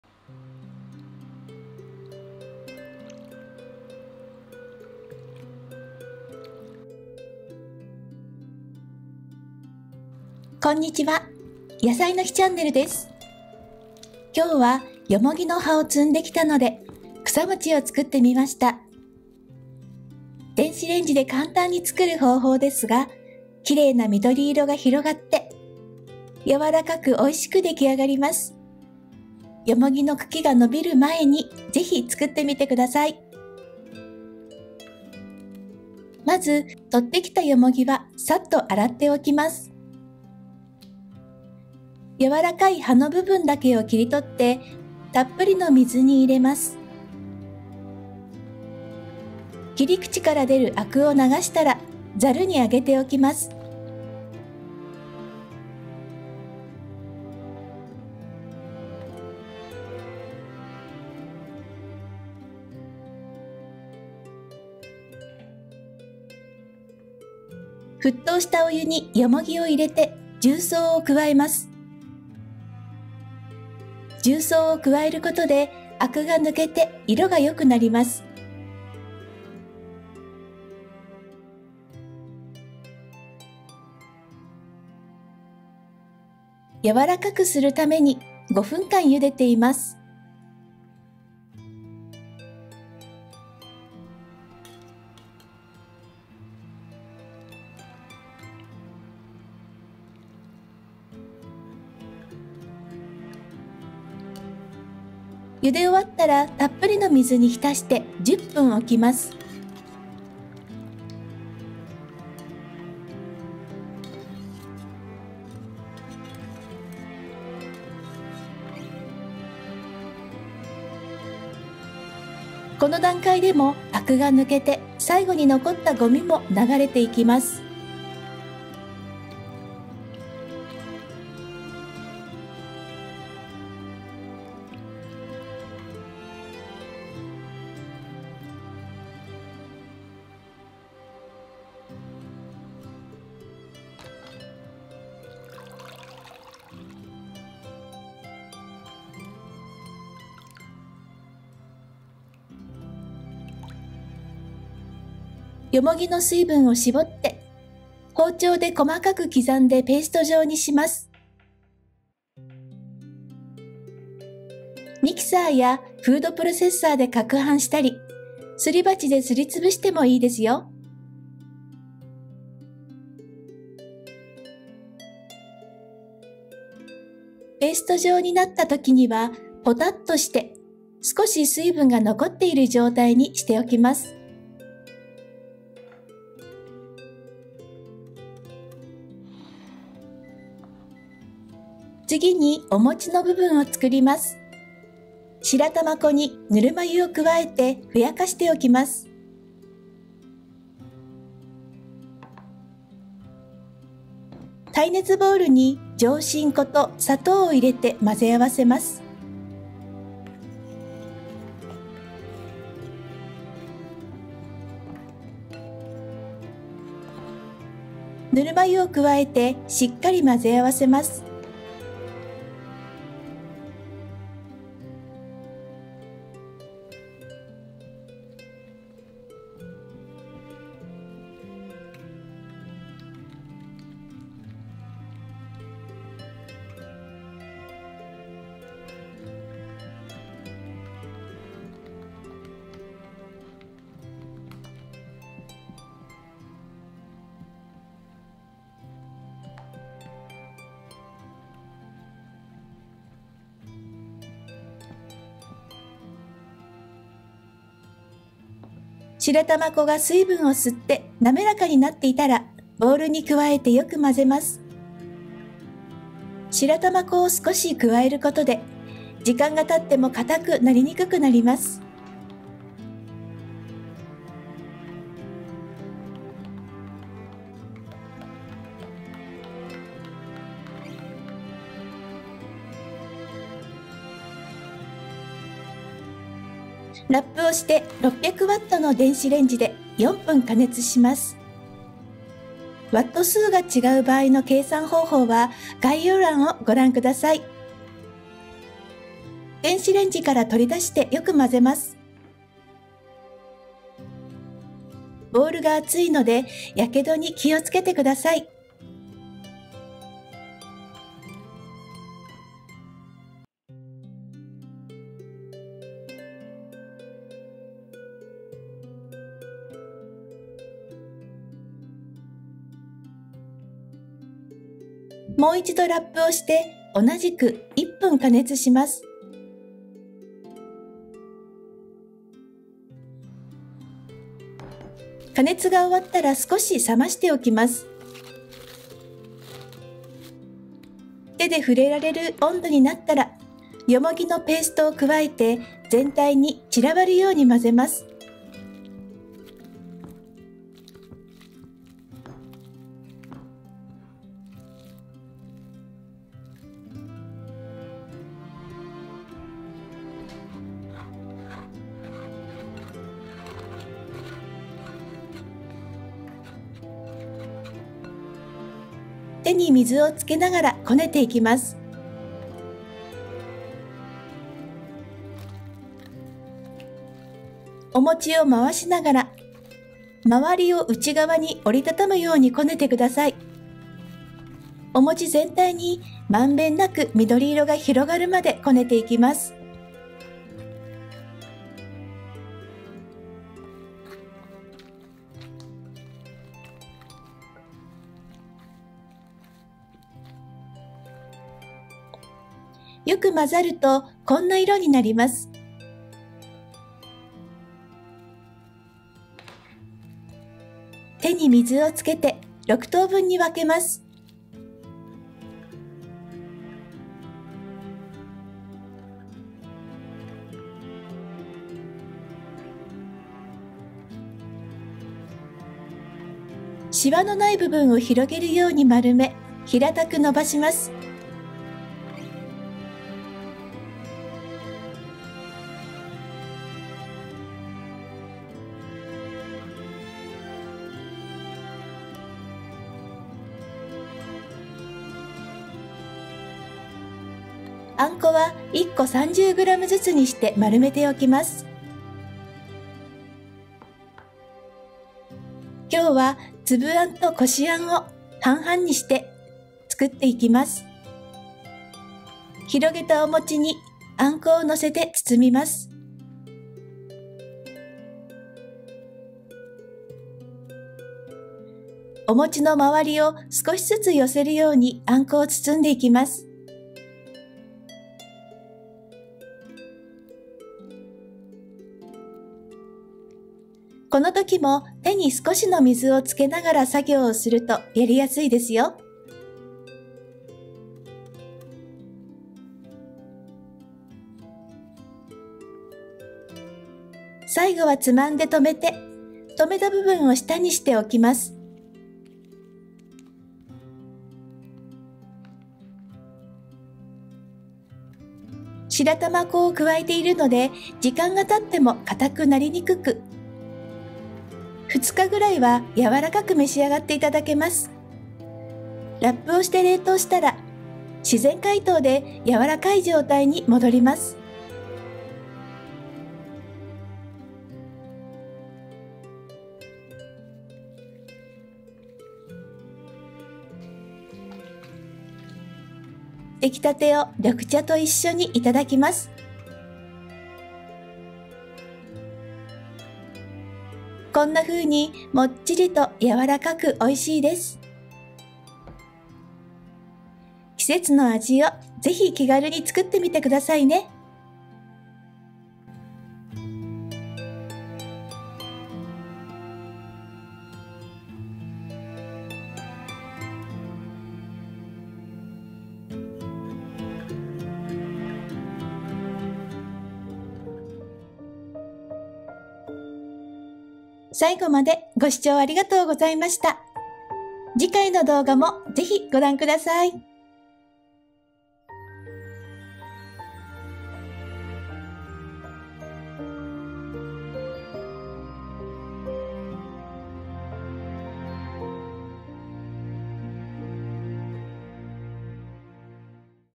（季節の和菓子）レンジで簡単！柔らかくよもぎの風味が美味しい「草餅」"Kusa mochi" Japanese sweets. How to make delicious （Subtitle)
今日はよもぎの葉を摘んできたので、草餅を作ってみました。
電子レンジで簡単に作る方法ですが、きれいな緑色が広がって柔らかく美味しく出来上がります。
よもぎの茎が伸びる前に、ぜひ作ってみてください。

草餅（よもぎ餅）
（材料）６個
よもぎの葉　約50g（ペーストにして35g）
重曹   小さじ1/2

白玉粉　20g
ぬるま湯　大さじ3
上新粉　80g
砂糖　10g
ぬるま湯　80g

６００Wの電子レンジで４分→１分

（電子レンジのW数が違う場合、こちらを基準にしてください）
５００W　2分30秒
６００W　2分
７００W　1分40秒
８００W　1分30秒
1000W 　1分10秒

Today, I am making the classic Japanese sweet "Kusa Mochi".
Pluck the soft mugwort leaves and boil them into a paste.
It's an easy way to make it in the microwave.A beautiful green color spreads on the mochi, making it soft and delicious.By adding Shiratamako, it does not harden over time. You can enjoy it softly for about 2 days.After wrapping and freezing, it will return to a soft state by natural thawing.Please try to make it before the stem of mugwort grows.Please feel free to make seasonal flavors.

Kusa mochi ( Mugwort rice cake)
(Ingredients) 6 pieces
50g mugwort leaf about (35g as paste)
1/2 teaspoon baking soda

20g shiratamako
3 tablespoons of warm water
80g joshinko (rice flour)
10g sugar 10g
80g warm water